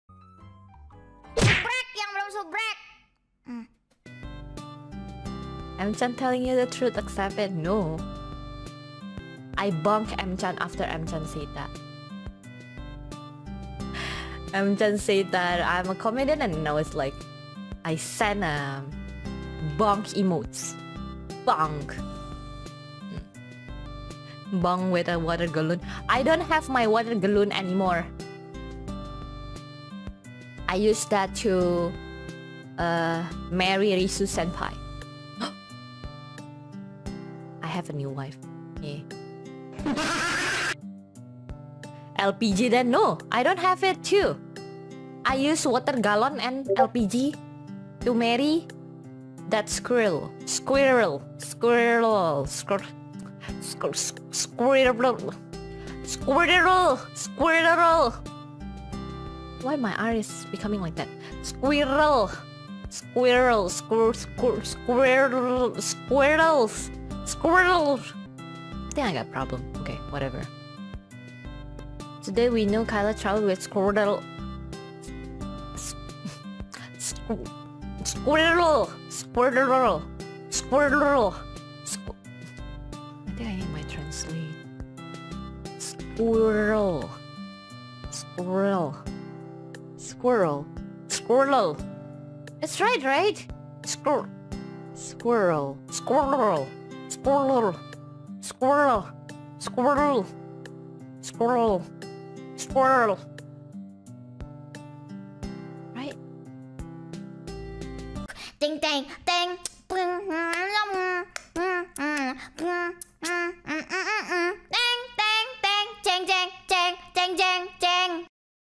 Kaela has a struggle to pronouce SQUIRREL and looks really cute [Hololive English Sub]
Hi guys